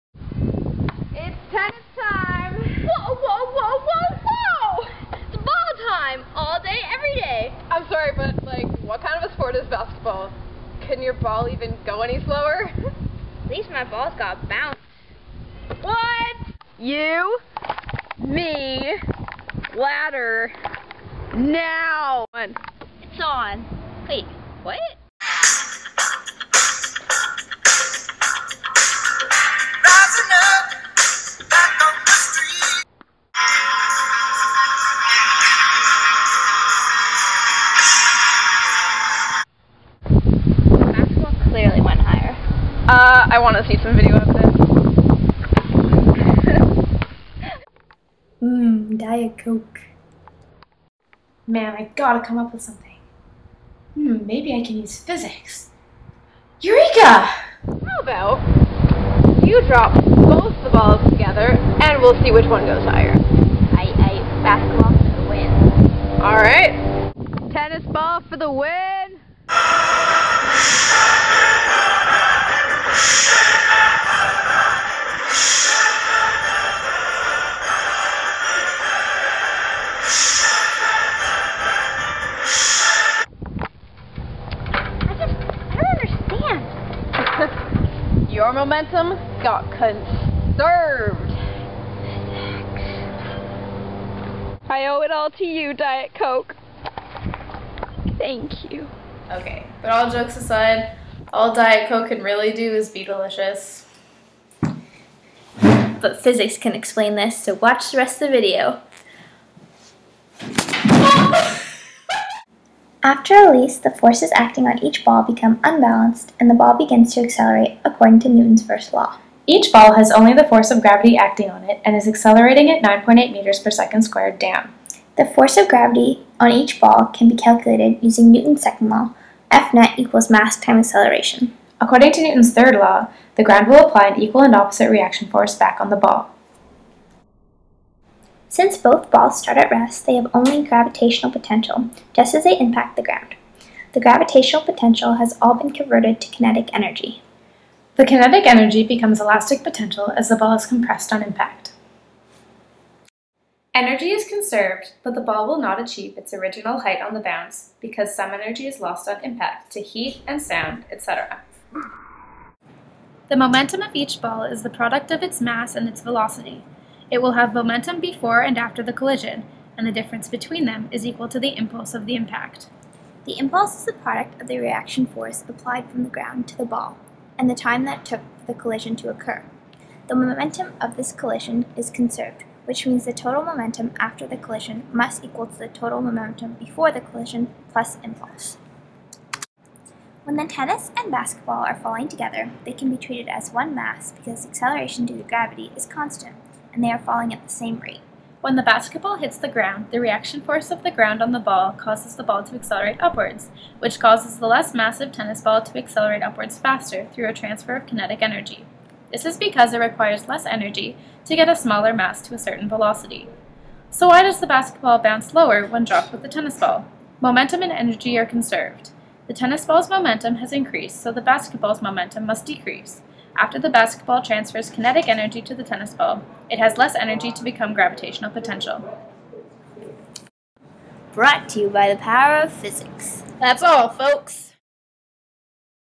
Double Ball Bounce
Grade 12 physics project explaining the physics concepts behind the double ball bounce. ENJOY